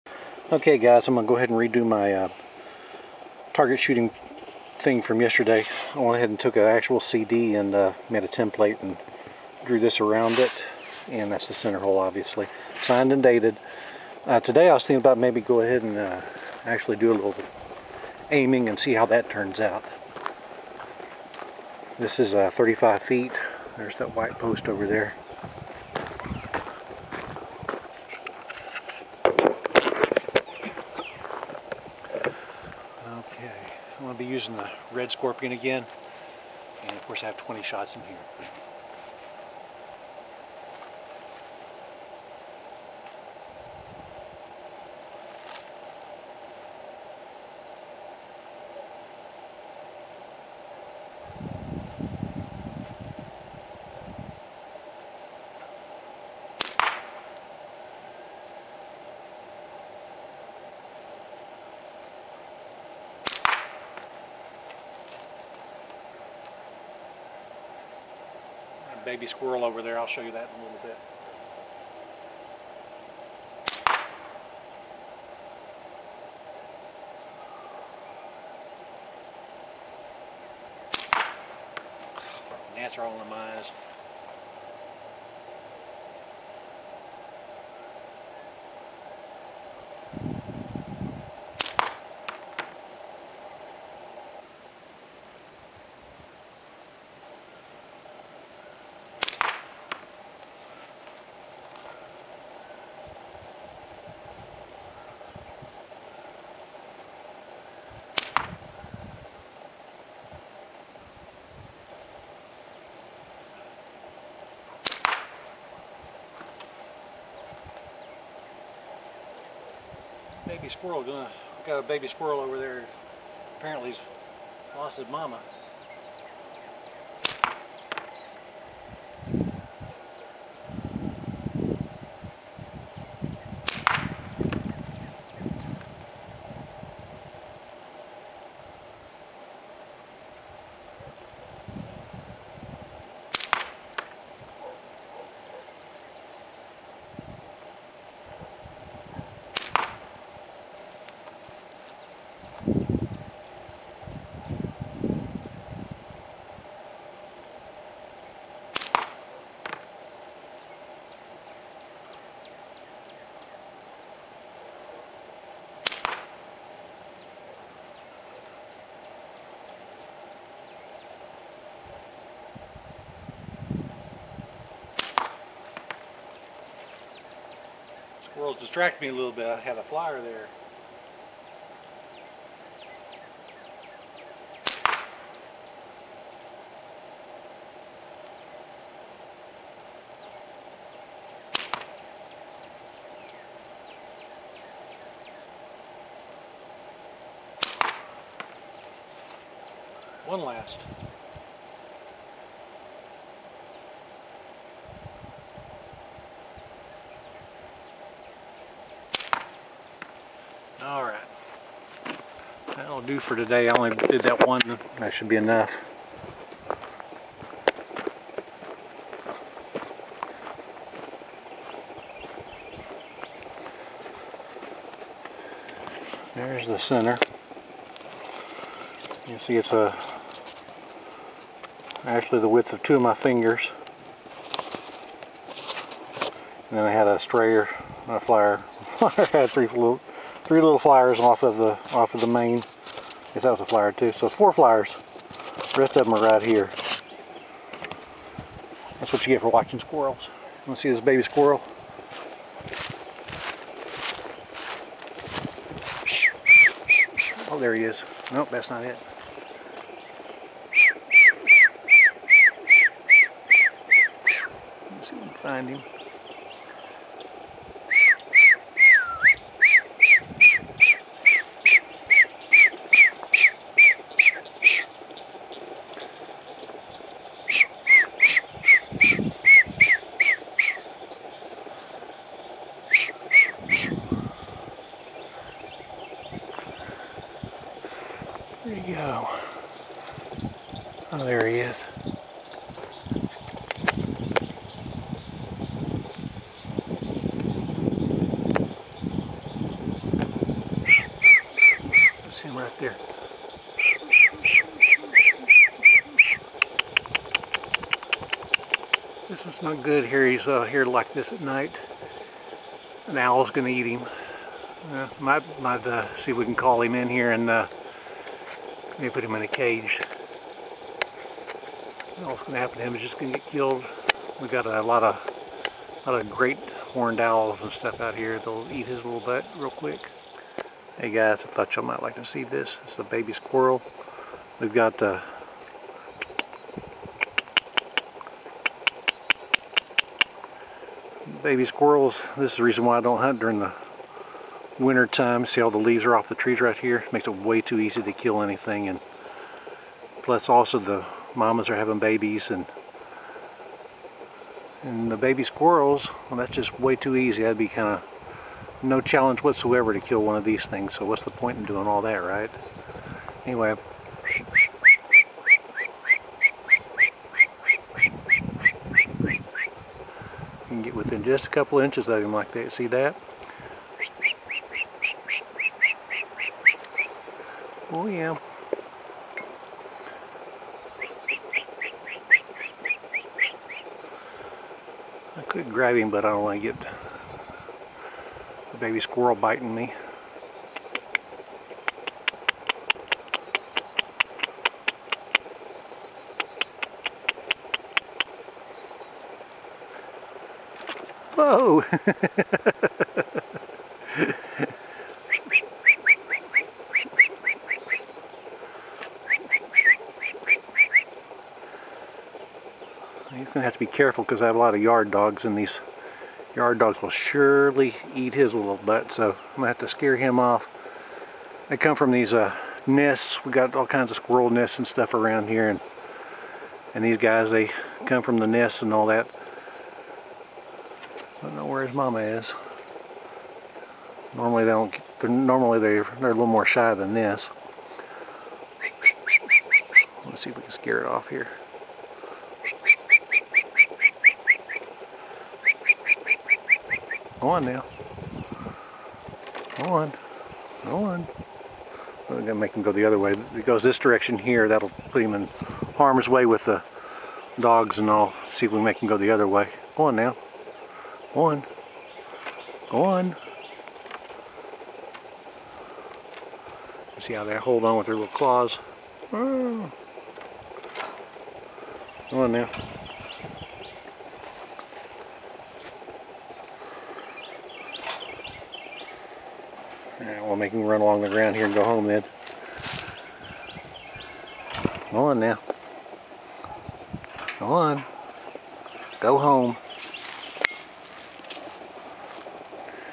Shooting for the Jorg Sprave forum global accuracy, plus a squirrel surprise at the end
PocketPredator.com The second video of slingshot shooting for the slingshot channel forum, global accuracy challenge.
Tonight I did try some... BUT a little squirrel kept distracting me. maybe I'll shoot again in a few days and do better then!